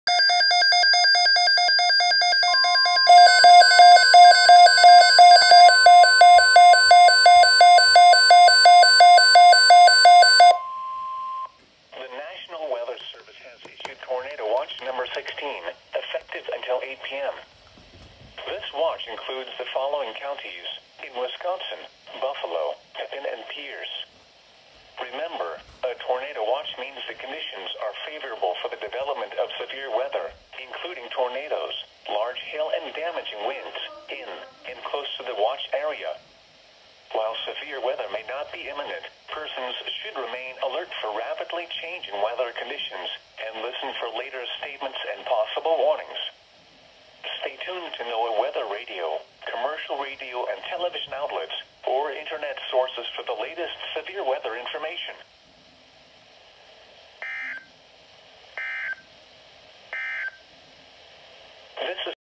Tornado Watch #16 [EAS #41]
URGENT - IMMEDIATE BROADCAST REQUESTED
   Tornado Watch Number 16
   NWS Storm Prediction Center Norman OK
   240 PM CST Wed Mar 10 2021

   The NWS Storm Prediction Center has issued a

   * Tornado Watch for portions of
     central and northeastern Iowa
     southeastern Minnesota
     a small portion of west central Wisconsin

   * Effective this Wednesday afternoon and evening from 240 PM
     until 800 PM CST.

   * Primary threats include...
     A couple tornadoes possible
     Scattered damaging wind gusts to 70 mph possible
     Scattered large hail events to 1.5 inches in diameter possible

   SUMMARY...Scattered severe thunderstorms are expected to develop by
   late this afternoon.  This may include a few supercells with the
   potential to produce tornadoes, before a broken squall line evolves
   and advances eastward with damaging wind gusts likely becoming the
   primary severe hazard this evening.

   The tornado watch area is approximately along and 70 statute miles
   east and west of a line from 25 miles north northeast of Rochester
   MN to 5 miles southeast of Des Moines IA. For a complete depiction
   of the watch see the associated watch outline update (WOUS64 KWNS
   WOU6).

   PRECAUTIONARY/PREPAREDNESS ACTIONS...

   REMEMBER...A Tornado Watch means conditions are favorable for
   tornadoes and severe thunderstorms in and close to the watch
   area. Persons in these areas should be on the lookout for
   threatening weather conditions and listen for later statements
   and possible warnings.